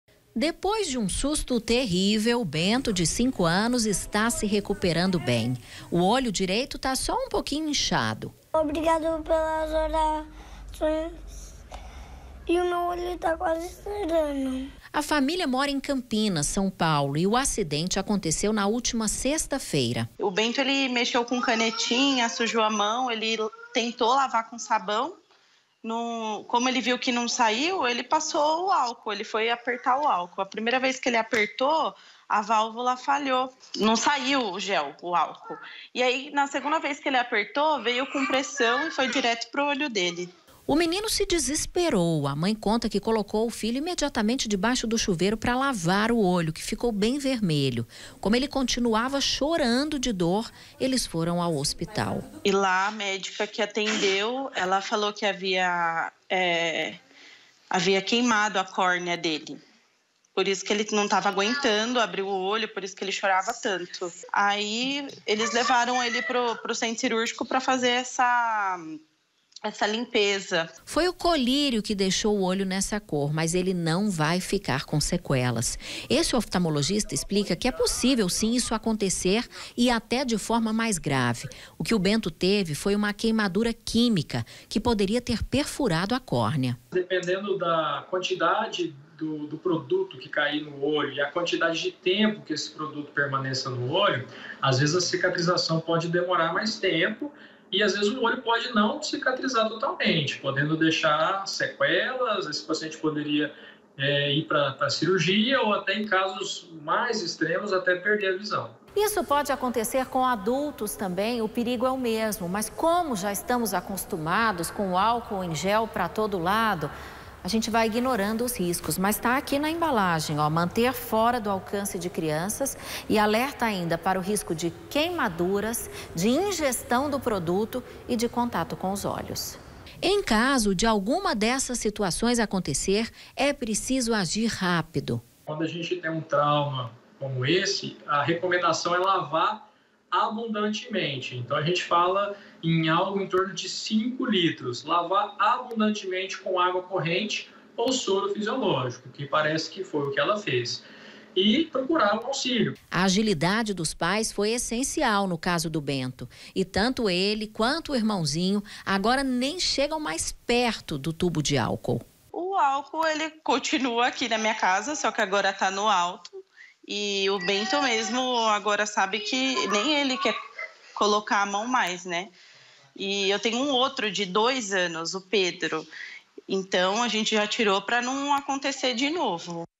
ÁLCOOL EM GEL CUIDADO COM OS OLHOS
O álcool em gel saiu com muita pressão do frasco e atingiu em cheio o olho da criança. É preciso ter cuidado, isso pode acontecer com qualquer pessoa.